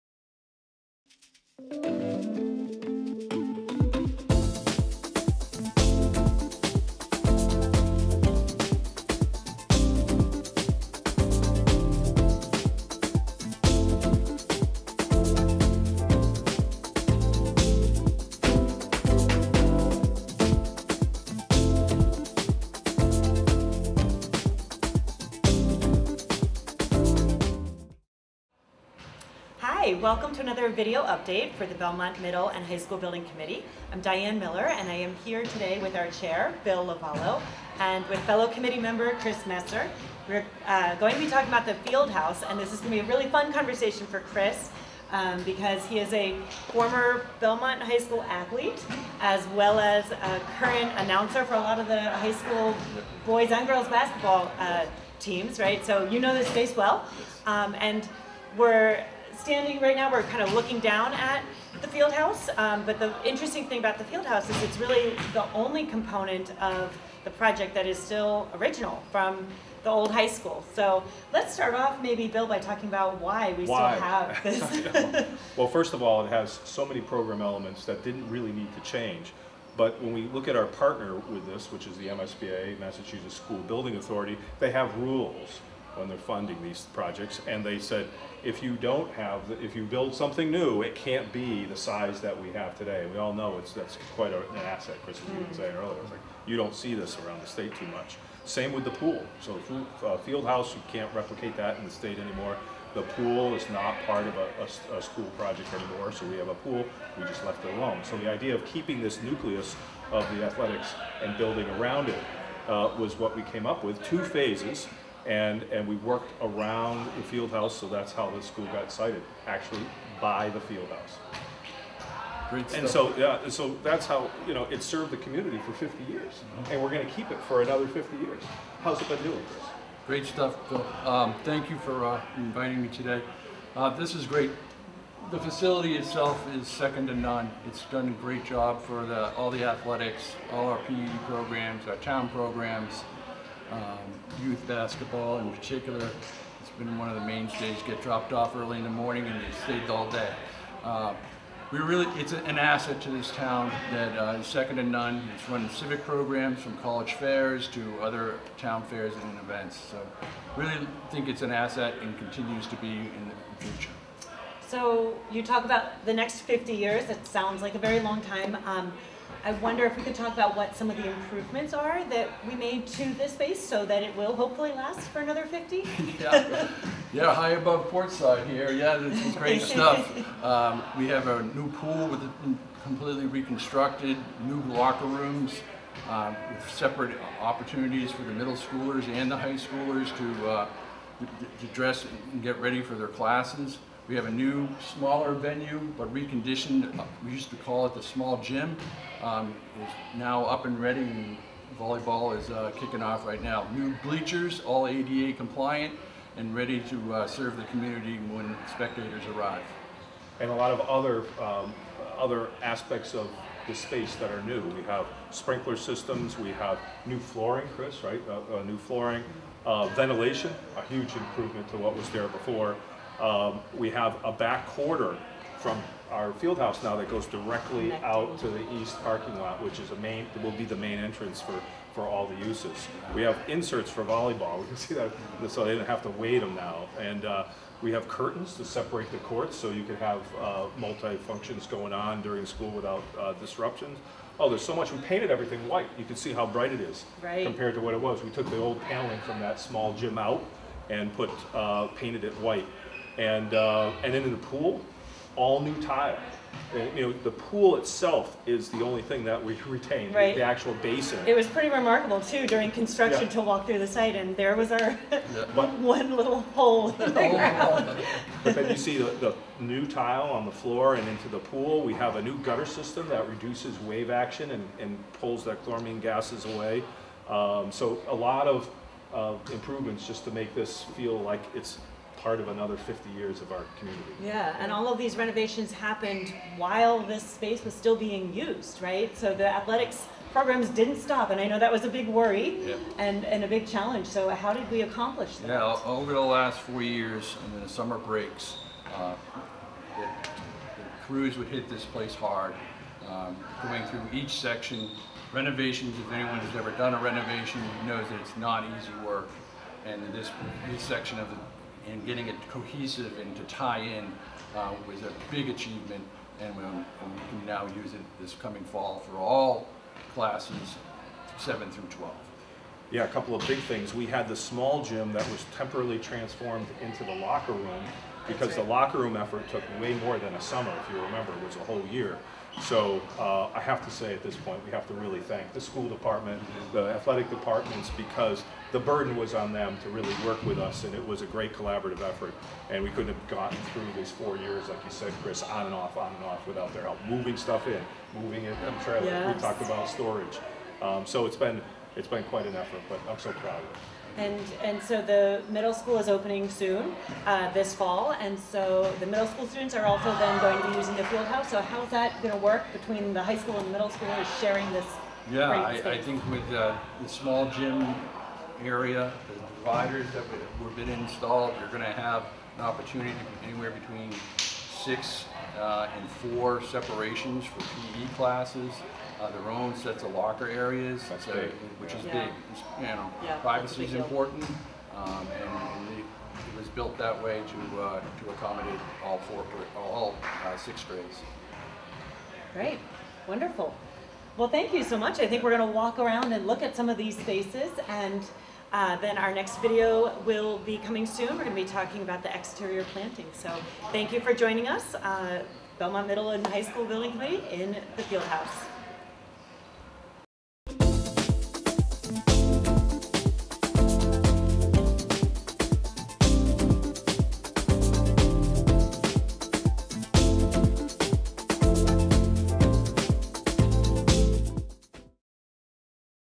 BMHS Video Update #12: Athletic Facilities, 7/23/23
Diane & Bill discuss the new athletic facilities with BMHS Building Committee member Chris Messer.